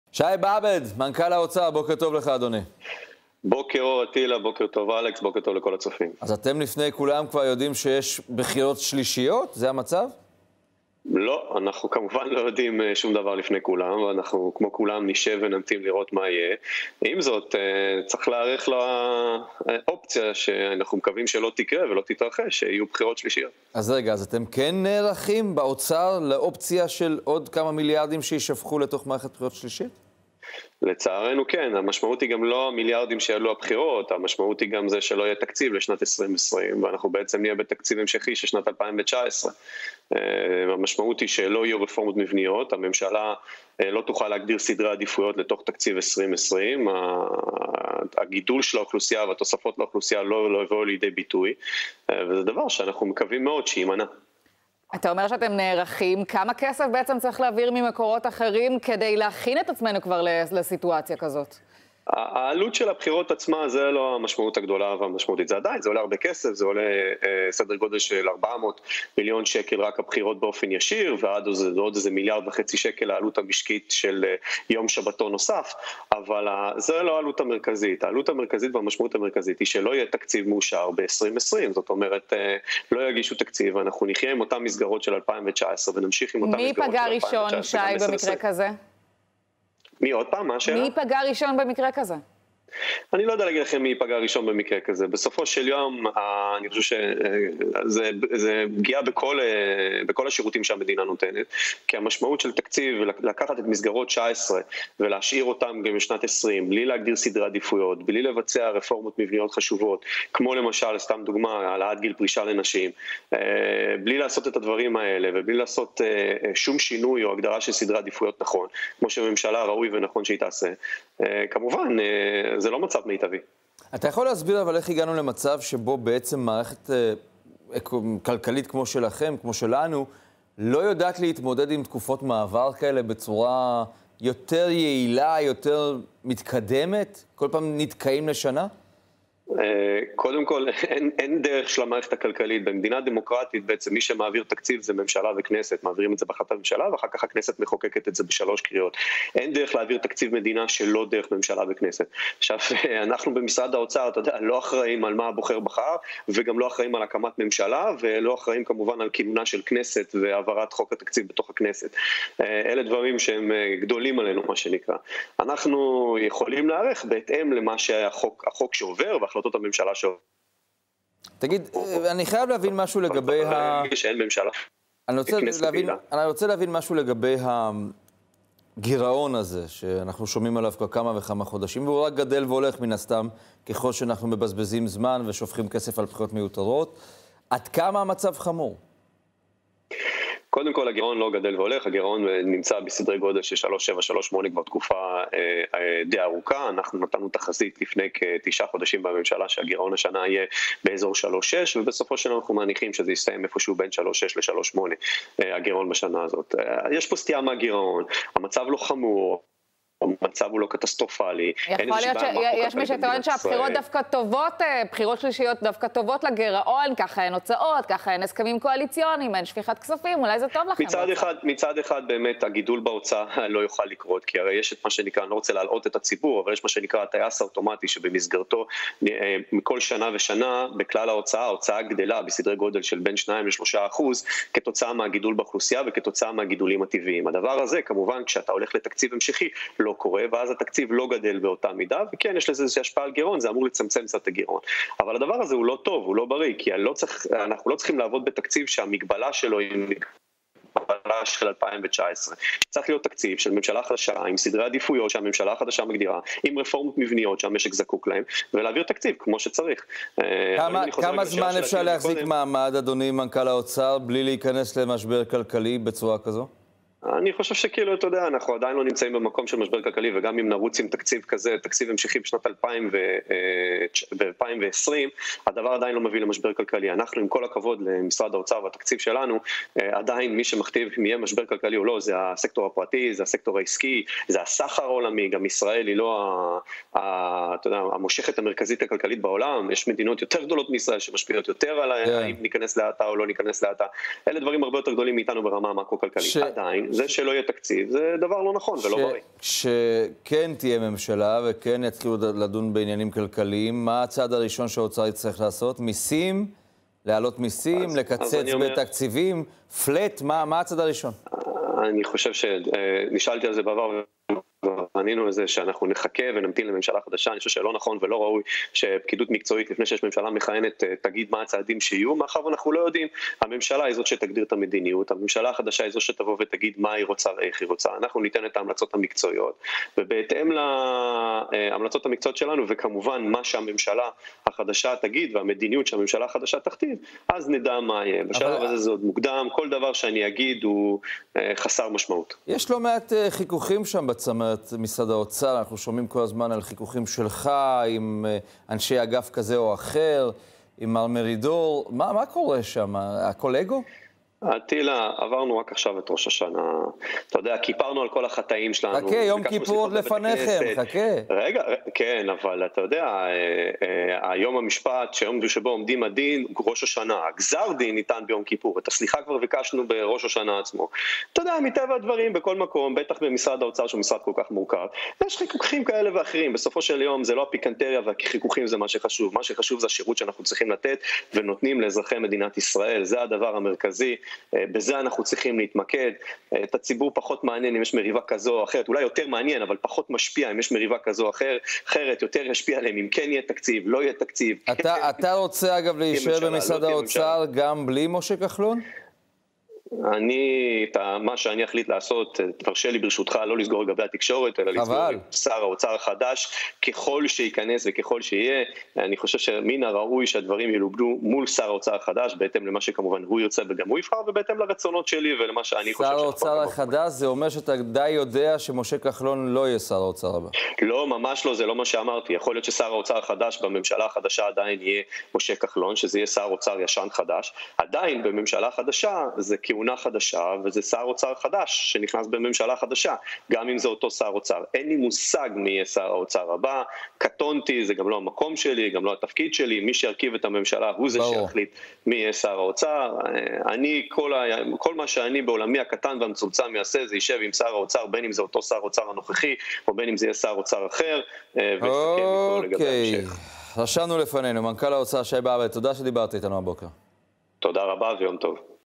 ראיון עם מנכ"ל משרד האוצר, שי באב"ד
מנכ"ל האוצר: "אם לא יהיה תקציב ל-2020 תהיה פגיעה בשירותי המדינה"